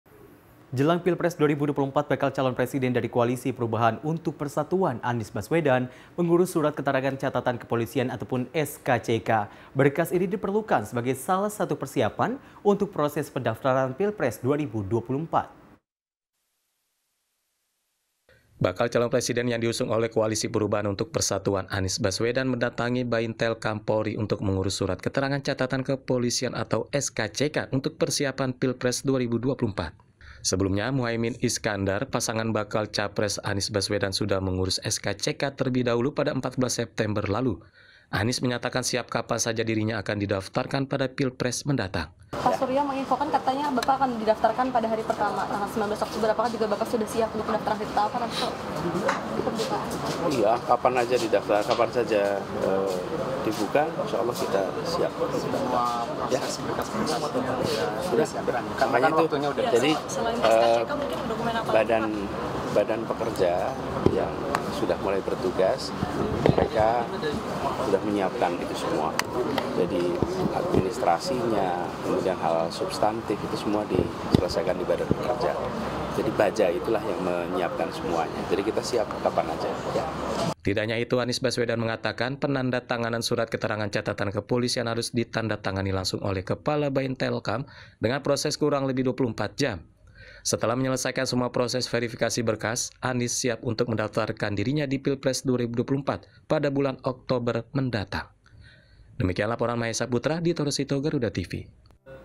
Anies Baswedan Mulai Urus Dokumen Untuk Daftar Pilpres 2024
Bakal calon presiden yang diusung oleh Koalisi Perubahan untuk Persatuan, Anies Baswedan, telah mengunjungi Baintelkam Polri untuk mengurus Surat Keterangan Catatan Kepolisian (S-K-C-K) sebagai bagian dari persiapannya menghadapi Pilpres 2024.

Sebelumnya, pada tanggal 14 September 2023, pasangan bakal calon presiden Anies Baswedan dan Muhaimin Iskandar sudah mengurus S-K-C-K. Anies Baswedan menyatakan kesiapannya untuk didaftarkan kapan saja pada Pilpres mendatang.

Anies Baswedan juga menekankan bahwa penandatanganan S-K-C-K harus dilakukan langsung oleh Kepala Baintelkam Polri dengan proses yang diharapkan berlangsung kurang lebih 24 jam. Setelah menyelesaikan semua proses verifikasi berkas, Anies siap untuk mendaftarkan dirinya dalam Pilpres 2024 pada bulan Oktober mendatang.